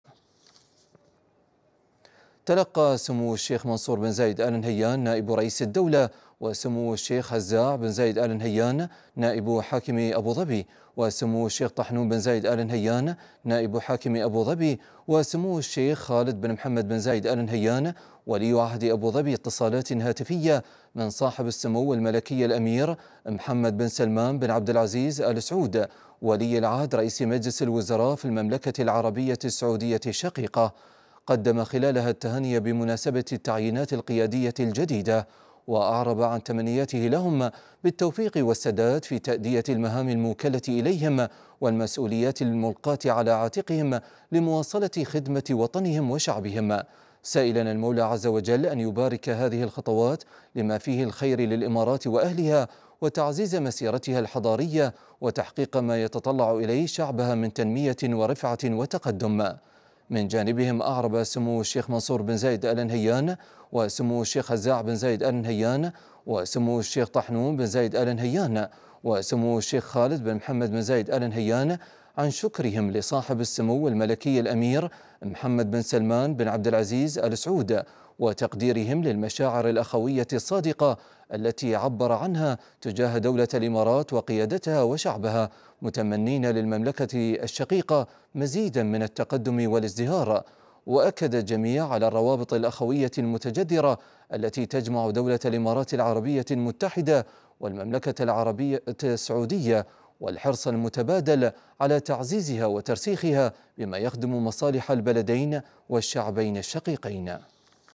منصور و هزاع وطحنون بن زايد و خالد بن محمد يتلقون تهاني محمد بن سلمان بمناسبة التعينات القيادية
أخبار الدار | منصور و هزاع وطحنون بن زايد و خالد بن محمد يتلقون تهاني محمد بن سلمان بمناسبة التعينات القيادية